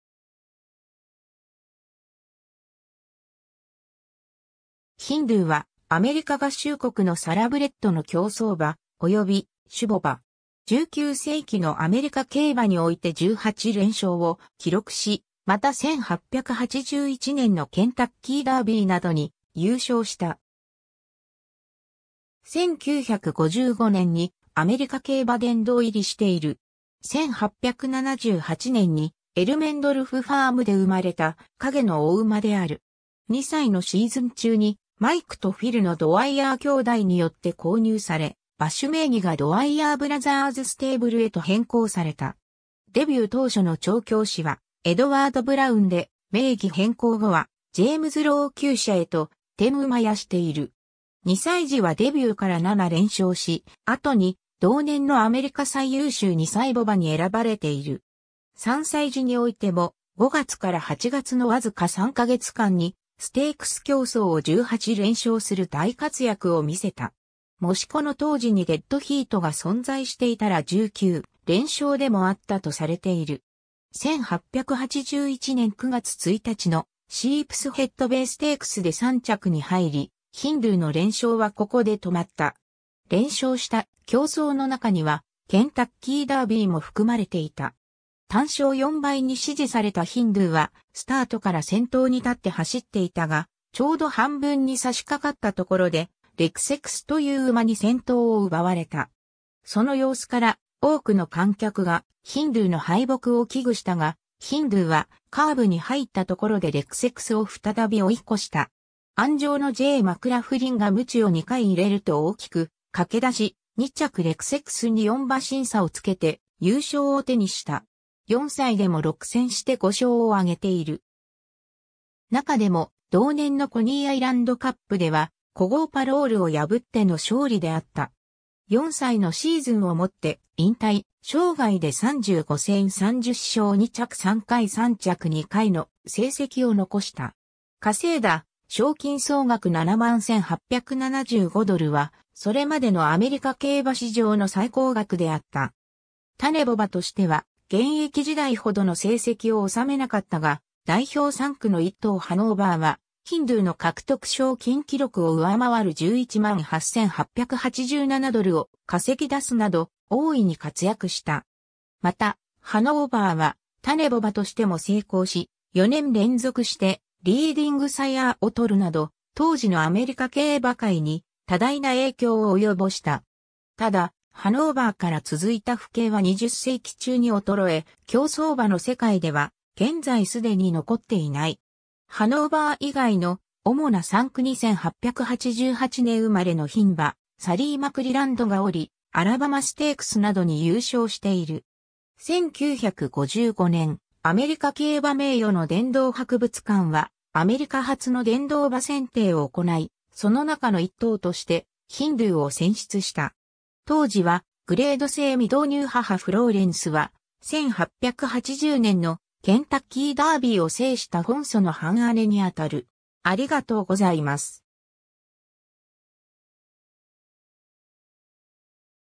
ヒンドゥー (競走馬)
ヒンドゥー（Hindoo、1878年 - 1901年）はアメリカ合衆国のサラブレッドの競走馬、および種牡馬。
19世紀のアメリカ競馬において18連勝を記録し、また1881年のケンタッキーダービーなどに優勝した。
1955年にアメリカ競馬殿堂入りしている。
 1878年にエルメンドルフファームで生まれた、鹿毛の牡馬である。
2歳のシーズン中に、マイクとフィルのドワイヤー兄弟によって購入され、馬主名義がドワイヤーブラザーズステーブルへと変更された。
デビュー当初の調教師はエドワード・ブラウンで、名義変更後はジェームズ・ロウ厩舎へと転厩している。
 2歳時はデビューから7連勝し、後に同年のアメリカ最優秀2歳牡馬に選ばれている。
3歳時においても、5月から8月のわずか3ヶ月間にステークス競走を18連勝する大活躍を見せた。
もしこの当時にデッドヒート（同着）が存在していたら19連勝でもあったとされている。
1881年9月1日のシープスヘッドベイステークスで3着に入り、ヒンドゥーの連勝はここで止まった。
 連勝した競走の中には、ケンタッキーダービーも含まれていた。
単勝4倍に支持されたヒンドゥーは、スタートから先頭に立って走っていたが、ちょうど半分に差し掛かったところでレクセクスという馬に先頭を奪われた。
その様子から多くの観客がヒンドゥーの敗北を危惧したが、ヒンドゥーはカーブに入ったところでレクセクスを再び追い越した。
鞍上のJ. マクラフリンが鞭を2回入れると大きく駆け出し、2着レクセクスに4馬身差をつけて優勝を手にした。
 4歳でも6戦して5勝を挙げている。
なかでも、同年のコニーアイランドカップ（18ハロン・約3421メートル）では古豪パロールを破っての勝利であった。
4歳のシーズンをもって引退、生涯で35戦30勝・2着3回・3着2回の成績を残した。
稼いだ賞金総額71,875ドルは、それまでのアメリカ競馬史上の最高額であった。
 種牡馬としては現役時代ほどの成績を収めなかったが、代表産駒の1頭ハノーヴァーはヒンドゥーの獲得賞金記録を上回る118,887ドルを稼ぎ出すなど大いに活躍した。
また、ハノーヴァーは種牡馬としても成功し、4年連続してリーディングサイアーを取るなど当時のアメリカ競馬界に多大な影響を及ぼした。
ただ、ハノーヴァーから続いた父系は20世紀中に衰え、競走馬の世界では現在すでに残っていない。
ハノーヴァー以外の主な産駒に1888年生まれの牝馬サリーマクリランドがおり、アラバマステークスなどに優勝している。
 1955年、アメリカ競馬名誉の殿堂博物館はアメリカ初の殿堂馬選定を行い、そのなかの1頭としてヒンドゥーを選出した。
 ※当時はグレード制未導入 母フローレンスは、1880年のケンタッキーダービーを制したフォンソの半姉にあたる。